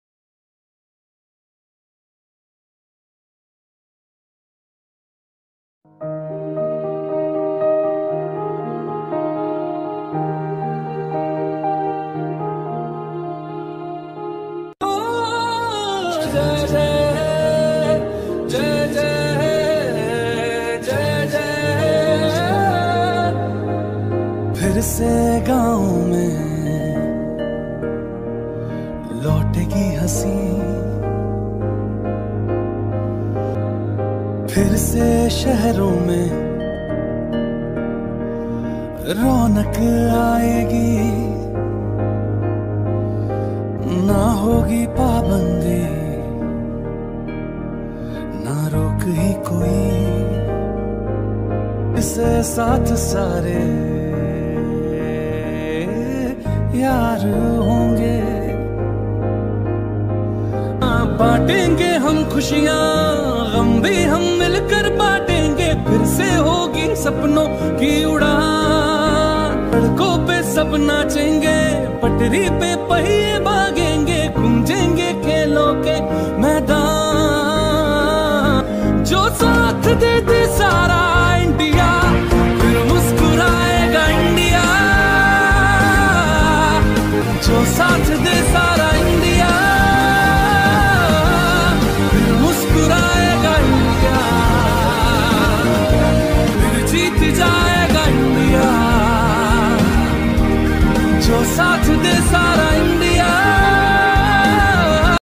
Awareness video on COVID19 by Bhatter College
NSS Unit of Bhatter College, Daton, West Bengal created an awareness video about general measures and precautions during COVID19.